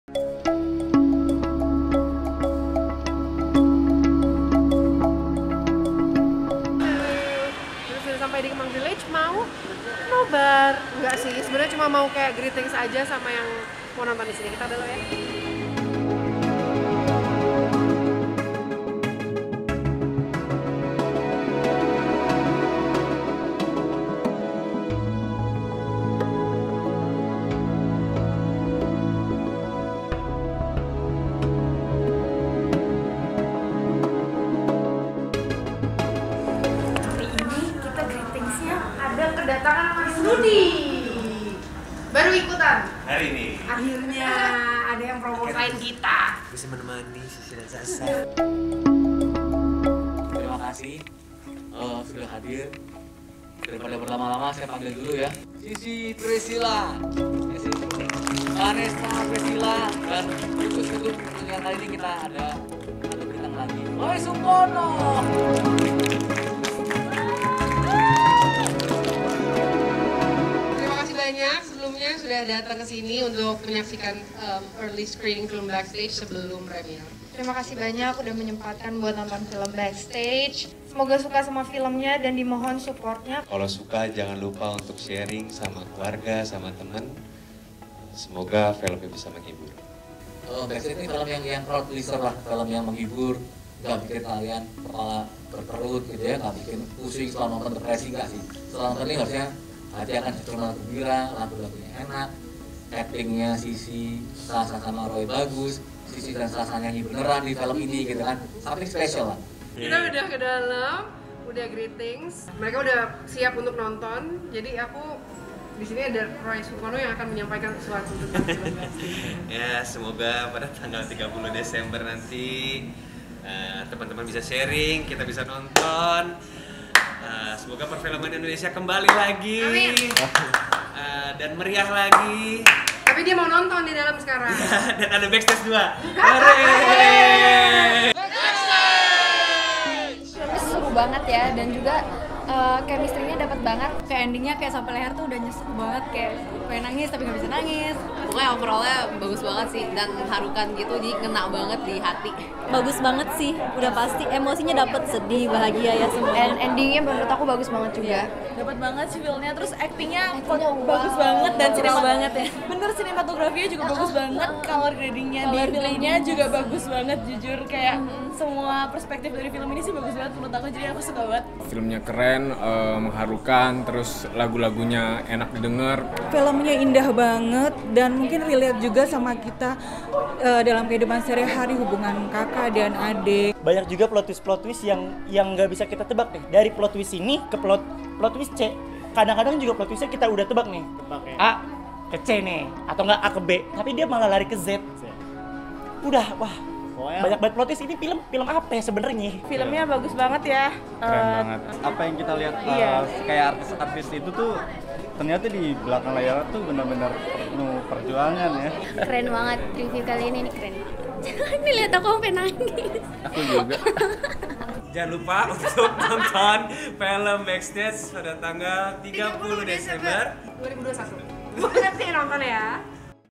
Backstage Menyapa Kemang Village | 30 Desember di Bioskop
Ikuti keseruan Sissy Prescillia dan Roy Sungkono nonton bareng di Kemang Village